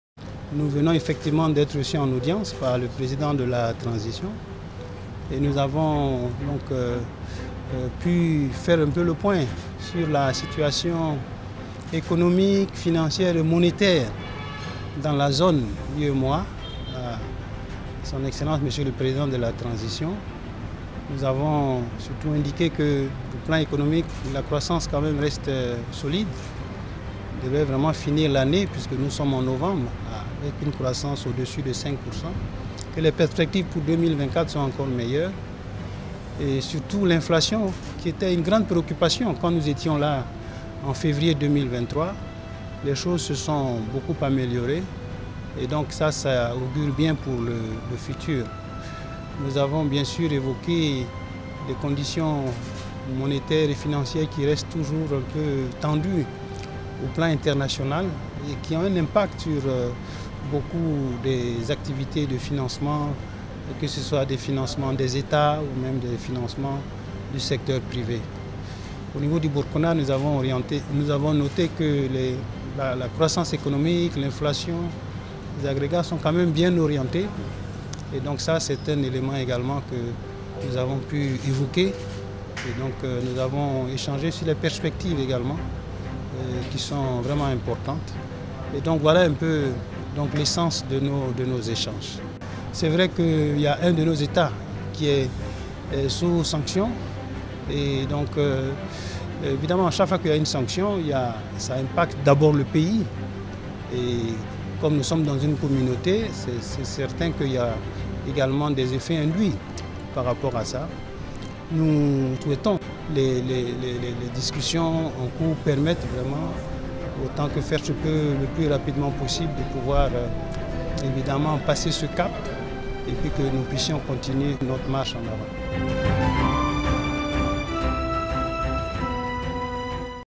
Situation économique dans la zone UEMOA : Le Gouverneur de la BCEAO fait le point au Chef de l’Etat
🔴 Le Gouverneur de la BCEAO, Jean-Claude Kassi BROU, évoque la santé économique dans la zone UEMOA, avec le Capitaine Ibrahim Traoré